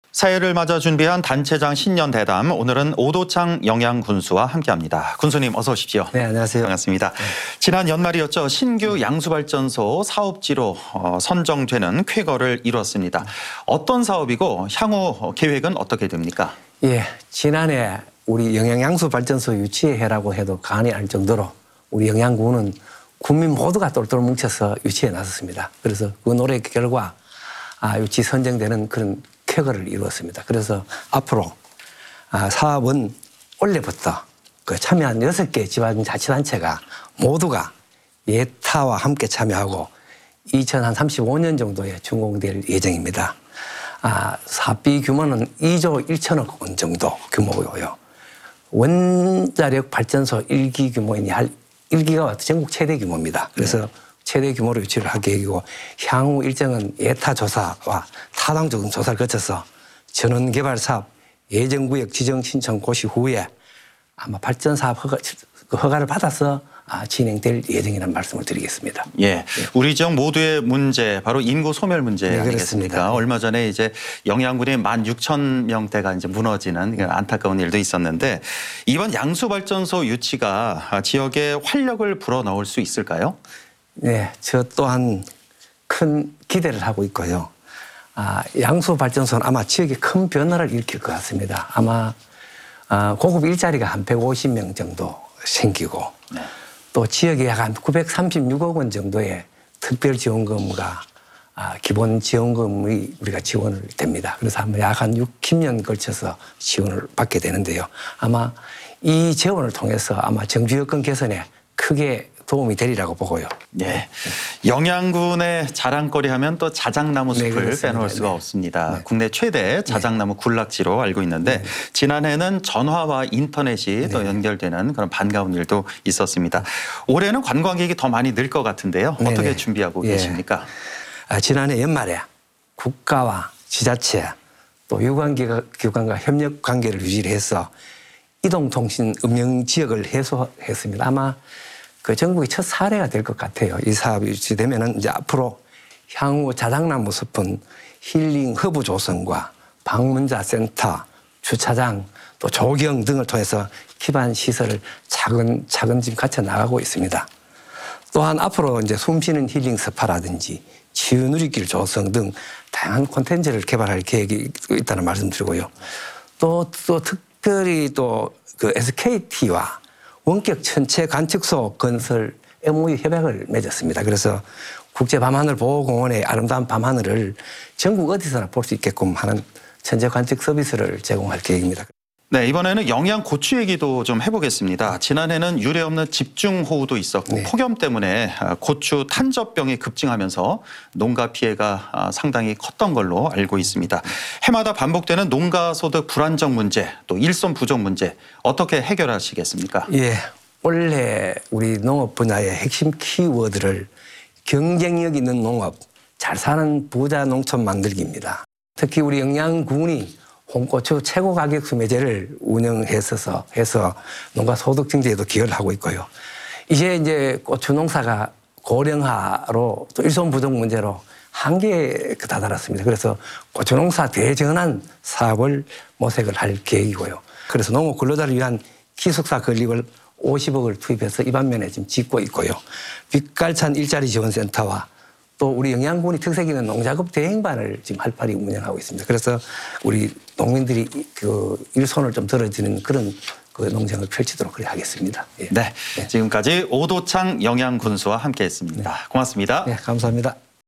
오도창 영양군수 "최대 규모 양수발전소로 지역 활력" / 안동MBC
2024/01/16 08:30:00    작성자 : 김경철

◀ 앵 커 ▶
새해를 맞아 준비한 단체장 신년 대담,
오늘은 오도창 영양군수와 함께합니다.

군수님, 어서오세요.

[질문 1]
지난 연말이었죠. 신규 양수발전소 사업지로
선정되는 쾌거를 이뤘습니다. 어떤 사업이고, 향후 계획은 어떻게 됩니까?

◀ INT ▶ 오도창 / 영양군수
"지난해 영양 양수발전소 유치의 해라고 해도 과언이 아닐 정도로 영양군은 군민 모두가
똘똘 뭉쳐서 유치에 나섰습니다. 그 노력의
결과 유치에 선정되는 쾌거를 이뤘습니다.

앞으로 사업은 올해부터 참여하는 6개
지방자치단체가 모두 예타와 함께 참여하고, 2035년 정도에 준공될 예정입니다.
사업비 규모는 2조 1천억 원 정도 규모이고요. 원자력 발전소 1기 규모인 1기가와트 전국 최대 규모입니다. 최대 규모를 유치할 계획이고.

향후 일정은 예타 조사와 타당성 조사를 거쳐서 전원개발사업 예정 구역 신청 고시 후에
발전사업 허가를 받아서 진행될 예정이라는
말씀을 드리겠습니다."

[질문 2]
우리 지역 모두의 문제, 바로 인구 소멸 문제 아니겠습니까? 얼마 전에 영양군이
1만 6천 명 대가 무너지는 안타까운 일도
있었는데, 이번 양수발전소 유치가
지역에 활력을 불어넣을 수 있을까요?

"저 또한 큰 기대를 하고 있고요. 양수발전소는 아마 지역에 큰 변화를 일으킬 것 같습니다.

아마 고급 일자리가 150개 정도 생기고,
또 지역에 936억 원 정도의 특별 지원금과
기본 지원금이 지원됩니다. 그래서 약 60년에 걸쳐서 지원을 받게 되는데요. 이 재원을
통해서 정주 여건 개선에 크게 도움이 되리라고 보고요.

[질문 3]
영양군의 자랑거리 하면 '자작나무숲'을 빼놓을 수가 없습니다. 국내 최대 자작나무 군락지로 알고 있는데, 지난해에는 전화와 인터넷이
연결되는 반가운 일도 있었습니다.
올해는 관광객이 더 많이 늘 것 같은데요.
어떻게 준비를 하고 있습니까?

"지난해 연말에 국가와 지자체, 유관기관과
협력 관계를 유지해서 이동통신 음영 지역을
해소했습니다. 아마 전국의 첫 사례일 것
같아요.

이 사업이 유치되면 앞으로 향후 자작나무 숲은 힐링 허브 조성과 방문자 센터, 주차장, 조경 등을 통해서 기반 시설을 차근차근 갖춰나가고 있습니다.

또한 앞으로 숨쉬는 힐링 스파라든지,
치유의 길 조성 등 다양한 콘텐츠를 개발할
계획이 있다는 말씀 드리고요.

또 특별히 SKT와 원격 천체 관측소 건설 MOU
협약을 맺었습니다. 그래서 국제 밤하늘
보호 공원의 아름다운 밤하늘을 전국 어디서나 볼 수 있게끔 하는 천체 관측 서비스를 제공할 계획입니다."

[질문 4]
이번에는 영양 고추 얘기도 해보겠습니다.
지난해에는 유례없는 집중호우도 있었고,
폭염 때문에 고추 탄저병이 급증하면서
농가 피해가 상당히 컸던 걸로 알고 있습니다. 해마다 반복되는 농가 소득 불안정 문제,
또 일손 부족 문제, 어떻게 해결하시겠습니까?

"올해 농업 분야의 핵심 키워드는 '경쟁력 있는 농업, 잘 사는 부자농촌 만들기'입니다.

특히 영양군이 홍고추 최고 가격 수매제를
운영해서 농가 소득 증대에도 기여를 하고
있고요.

이제 고추 농사가 고령화로, 또 일손 부족
문제로 한계에 다다랐습니다. 그래서
고추농사 대전환 사업을 모색할 계획이고요.

그래서 농업 근로자를 위한 기숙사 건립을
50억 원을 투입해서 입암면에 지금 짓고
있고요. 빛깔찬 일자리 지원 센터와 영양군의 특색 있는 농작업 대행반을 활발히 운영하고
있습니다. 그래서 농민들의 일손을 덜어드리는 그런 농정을 펼치도록 하겠습니다."

지금까지 오도창 영양군수와 함께 했습니다.
고맙습니다.